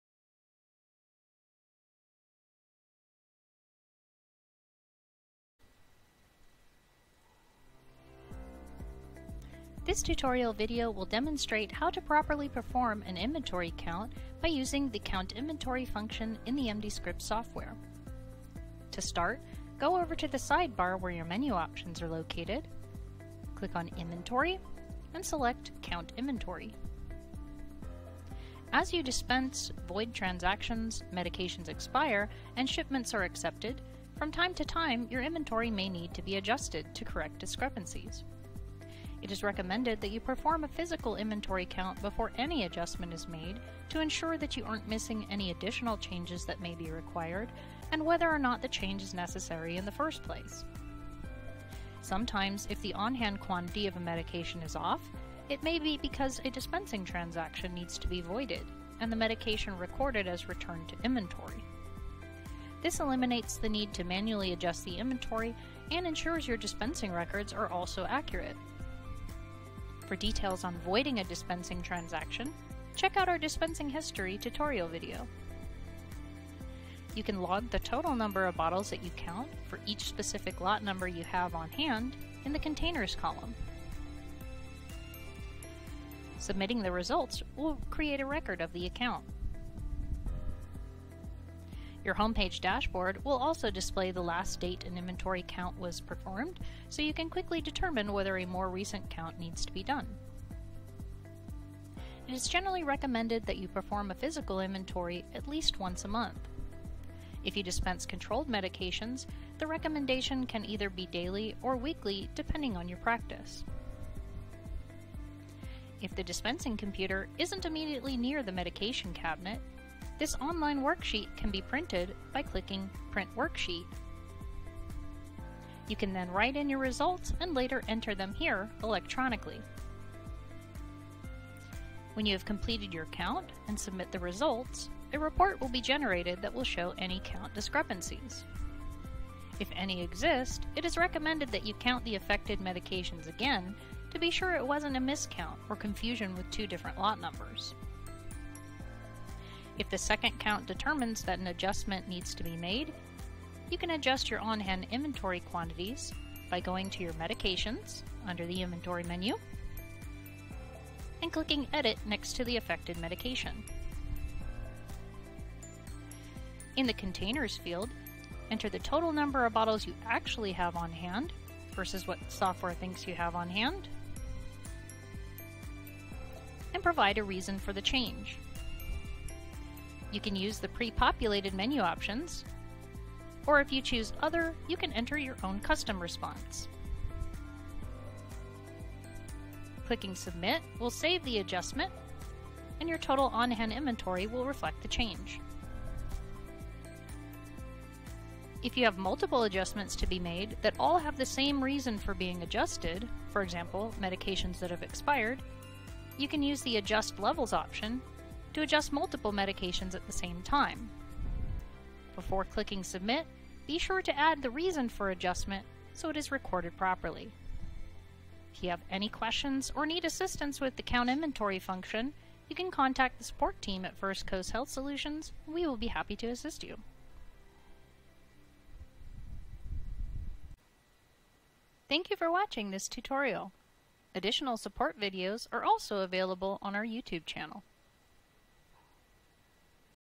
Counting and Adjusting Inventory
This tutorial video explains the steps to properly count and adjust inventory levels in the MDScripts software.